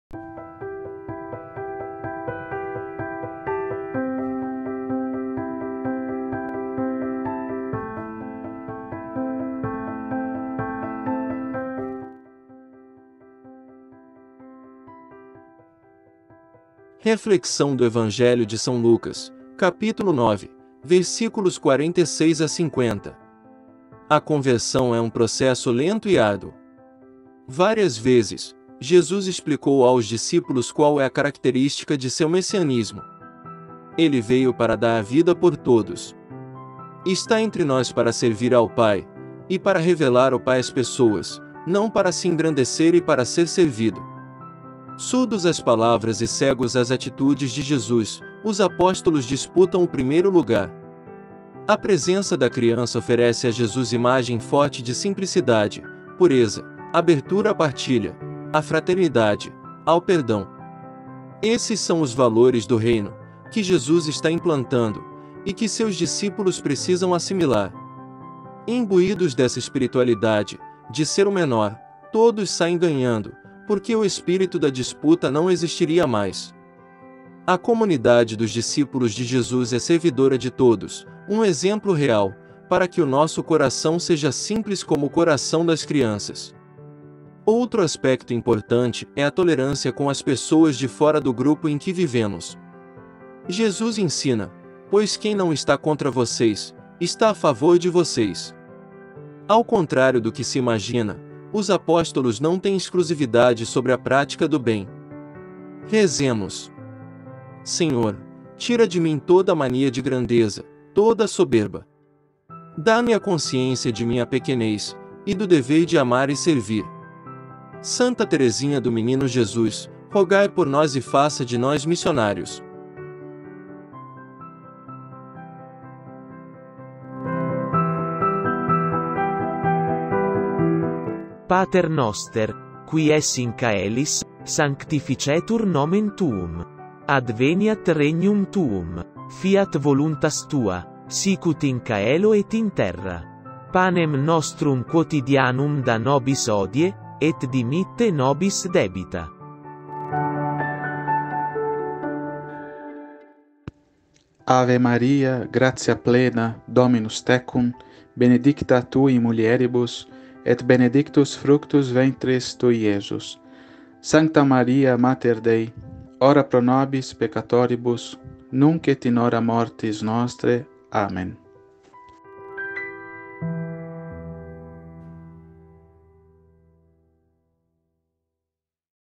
Reflexão da Liturgia Diária - Evangelho de São Lucas 9, 46-50
Naquele tempo:

Houve entre os discípulos uma discussão,
para saber qual deles seria o maior.

Jesus sabia o que estavam pensando.
Pegou então uma criança, colocou-a junto de si

e disse-lhes:
'Quem receber esta criança em meu nome,
estará recebendo a mim.
E quem me receber,
estará recebendo aquele que me enviou.
Pois aquele que entre todos vós for o menor,
esse é o maior.'

João disse a Jesus:
'Mestre, vimos um homem que expulsa demônios em teu nome.
Mas nós o proibimos, porque não anda conosco.'

Jesus disse-lhe:
'Não o proibais, pois quem não está contra vós,
está a vosso favor.'

Palavras relacionadas:
homilia diaria
homilia lucas 9
liturgia diaria
evangelho do dia
reflexão evangelho lucas 9